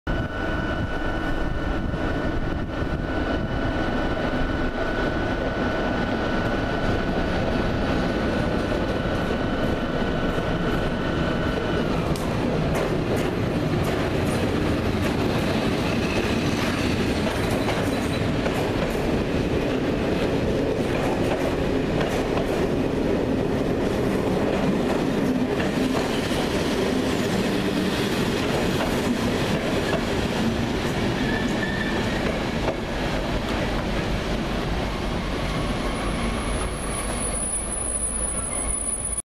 Train 7631 entering in Central Station Sofia
Fast Train 7631 entering in Sofia from Lom.